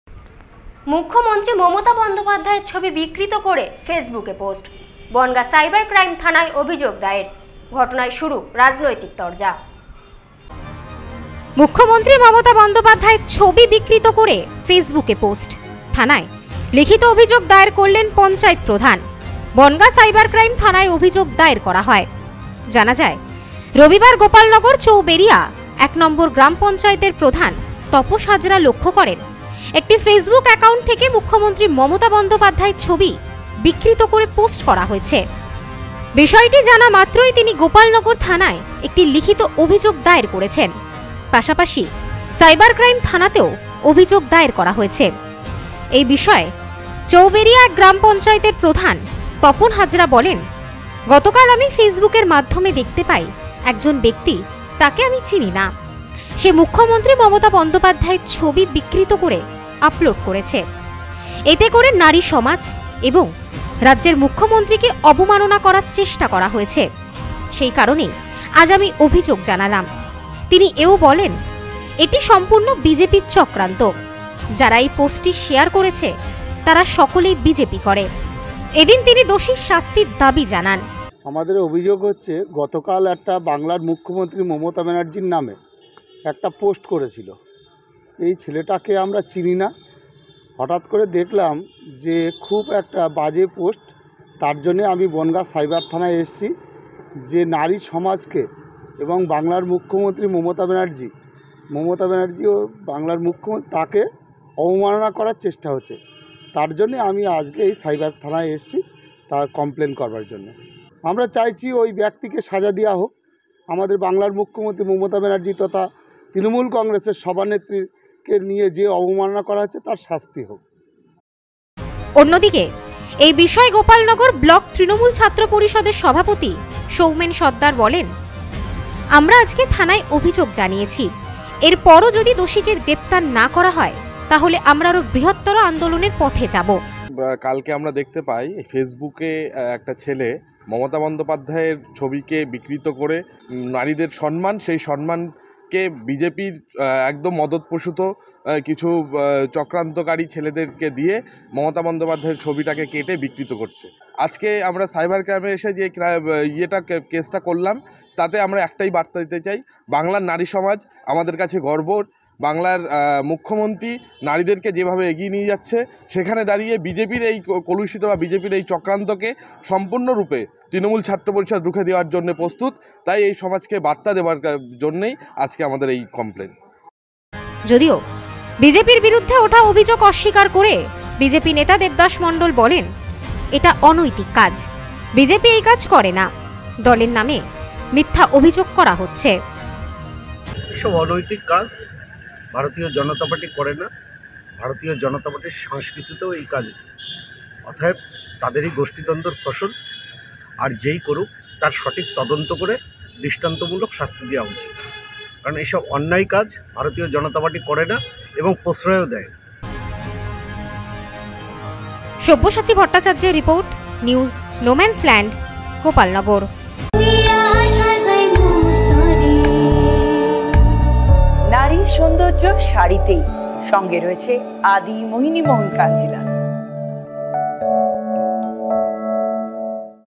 মুখ্যমন্ত্রীর বিকৃত ছবি ফেসবুকে! বনগাঁ সাইবার ক্রাইম থানায় অভিযোগ দায়ের
মুখ্যমন্ত্রী মমতা বন্দ্যোপাধ্যায়ের ছবি বিকৃত করে ফেসবুকে পোস্ট। বনগাঁ সাইবার ক্রাইম থানায় অভিযোগ দায়ের। ঘটনায় শুরু রাজনৈতিক তরজা।